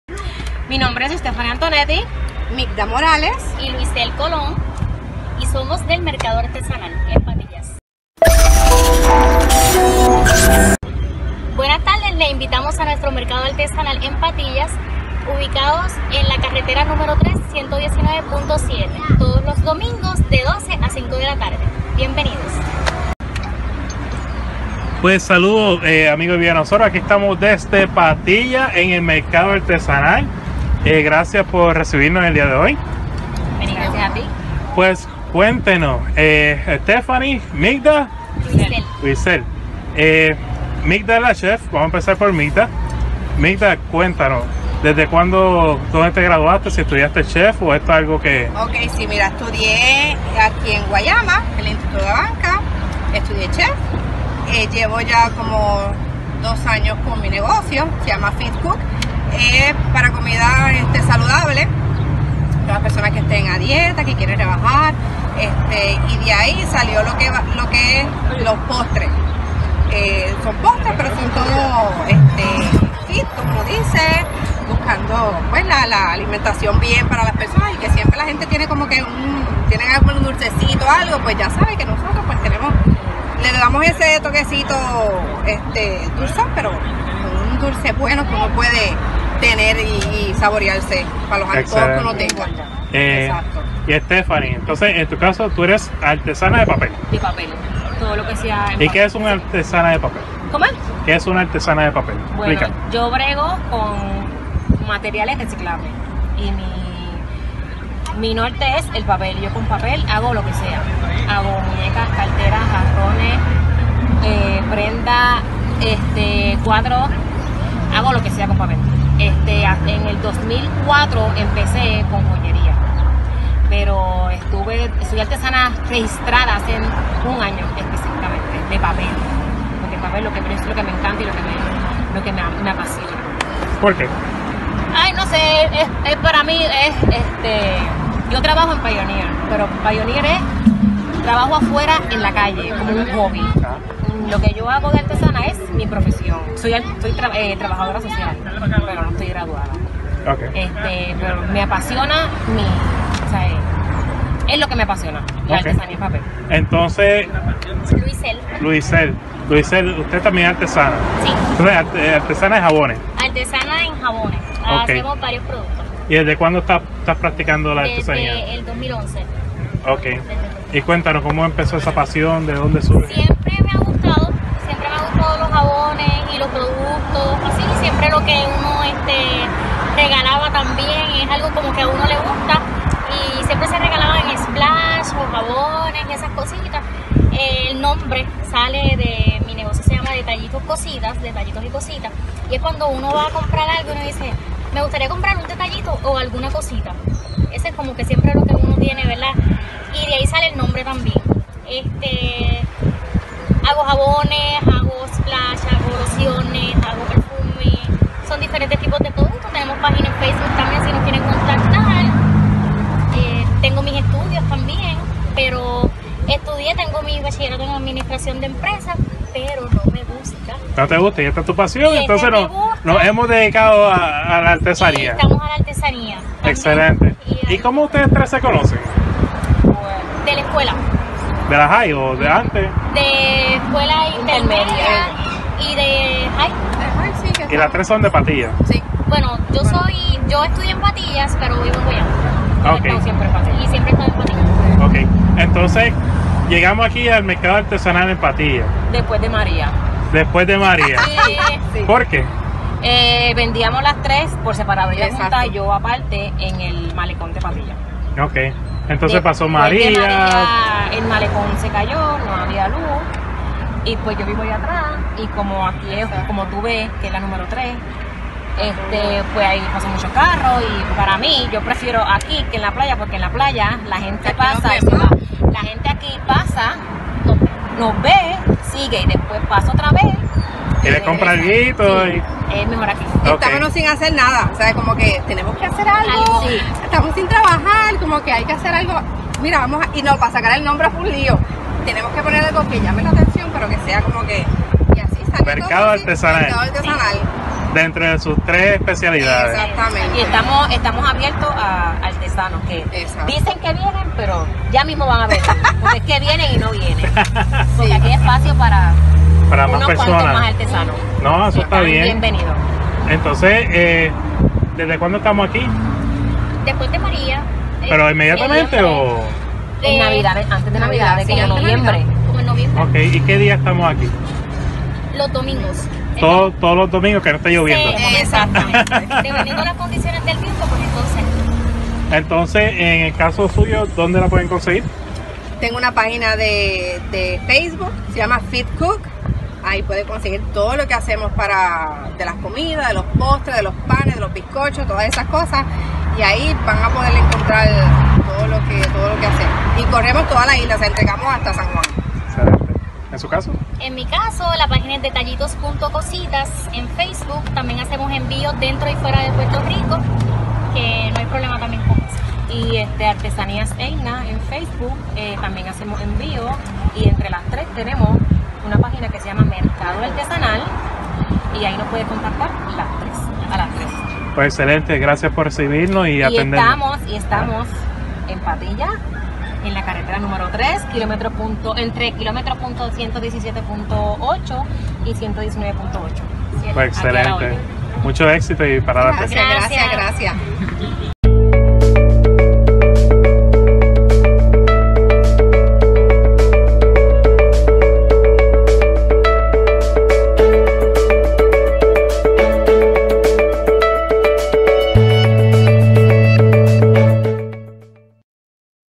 Mercado Artesanal en Patillas Puerto Rico
Hoy te presentamos 3 jovenes talentosas del Pueblo de Patillas. Ellas crearon lo que se conoce como el Mercado Artesanal de Patillas. Digno ejemplo de lucha y optimismo en Puerto Rico.